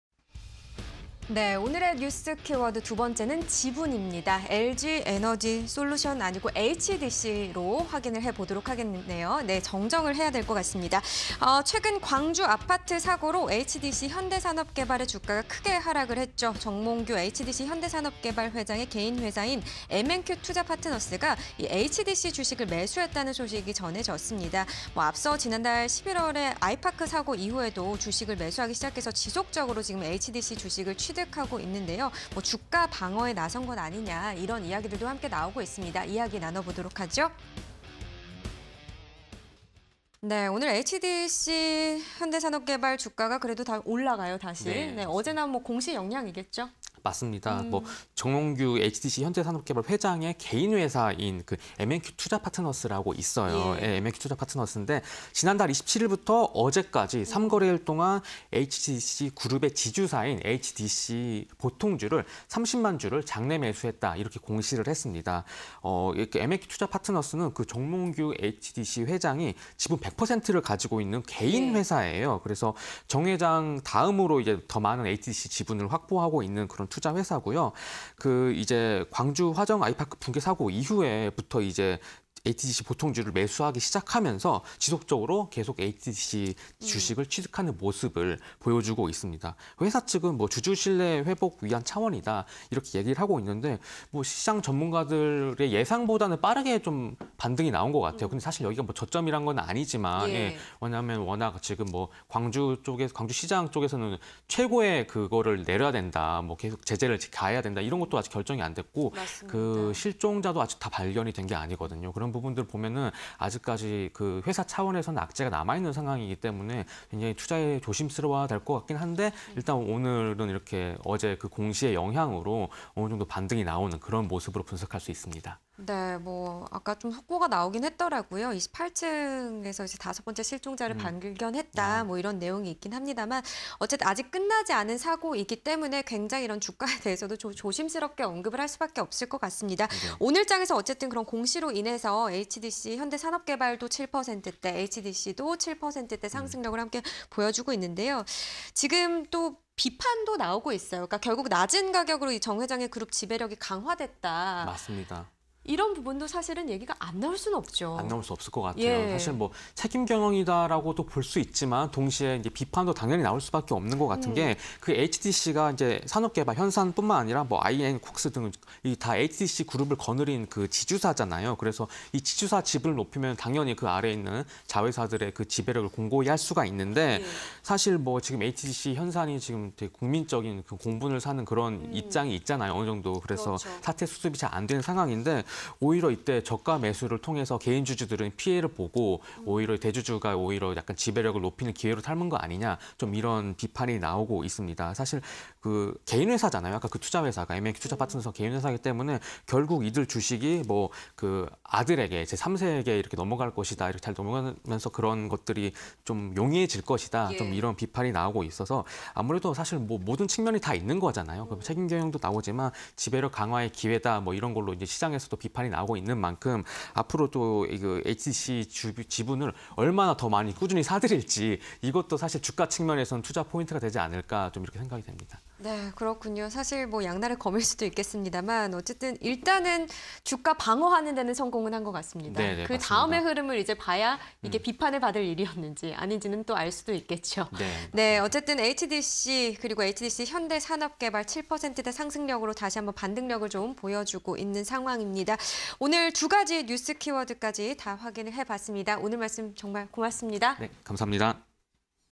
HDC(012630)... 정몽규 HDC현대산업개발 회장, HDC 지분 대거 확보_키워드 팩토리 (20220204)
HDC(012630)
- 정몽규 HDC현대산업개발 회장, HDC 주식 매수
- 엠엔큐투자파트너스, 정 회장이 지분 100% 보유
- 광주 붕괴 사고 이후 HDC 주식 매수 지속
- HDC, HDC현대산업개발·아이앤콘스 등 지주사
- 정몽규 회장, HDC 그룹 지배력 강화 예상
- 현재 HDC 주가, 지난해 5월 대비 폭락